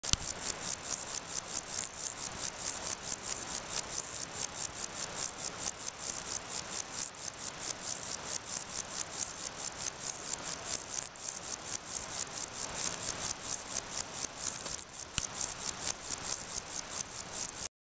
Metrioptera buyssoni - stridulation
Stridulation de la Decticelle ariégeoise (Metrioptera buyssoni), espèce endémique des Pyrénées.

Filmée par Pierre-Olivier Cochard le 15 août 2013, les Bordes-sur-Lez (Ariège), altitude 1600 m.